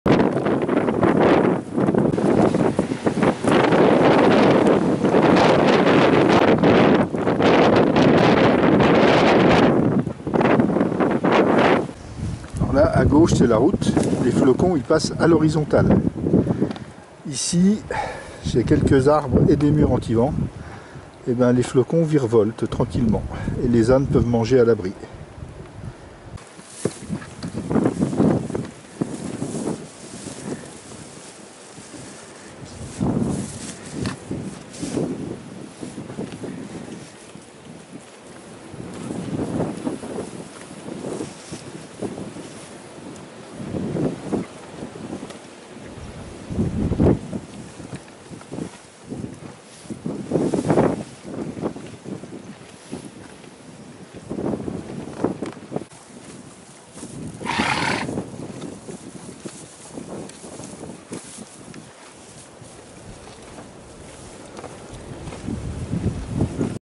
Les ânes dans la tempête 21 avril 2020
Quel mois d'avril ! Chaque fois qu'il fait beau, que l'on croit enfin le printemps arrivé, l'hiver revient en force.